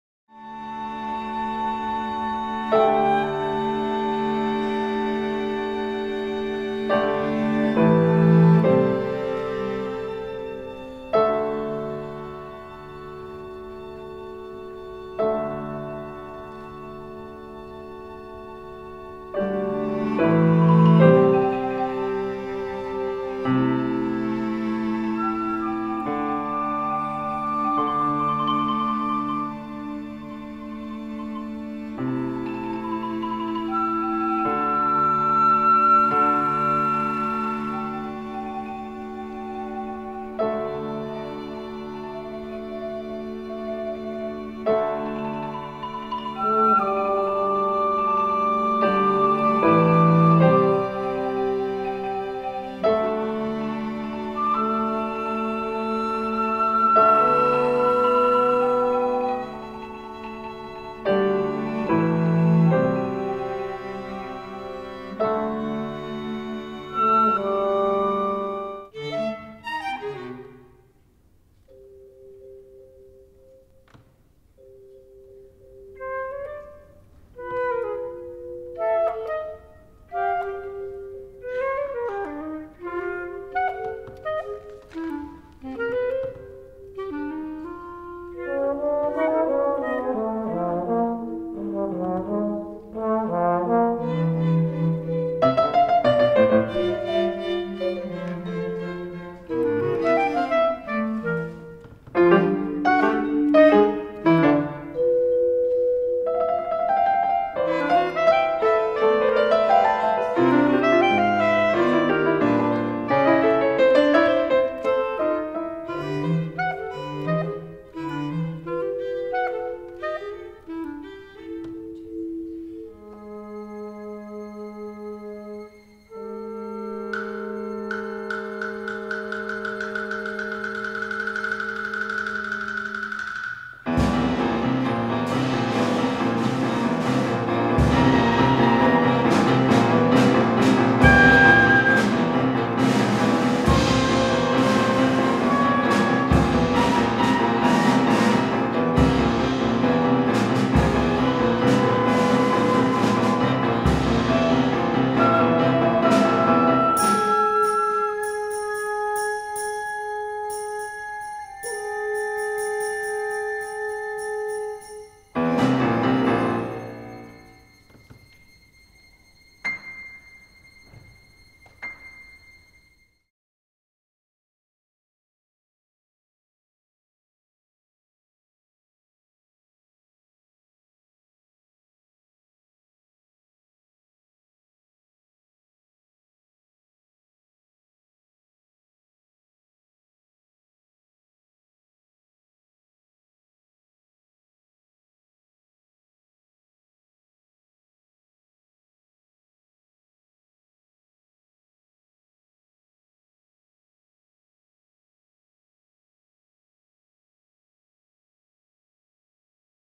Wits Music composition students: works for octet
Original compositions for octet by Wits Music final-year BMus composition students at the annual Sound Us Out student composition concert, 2013.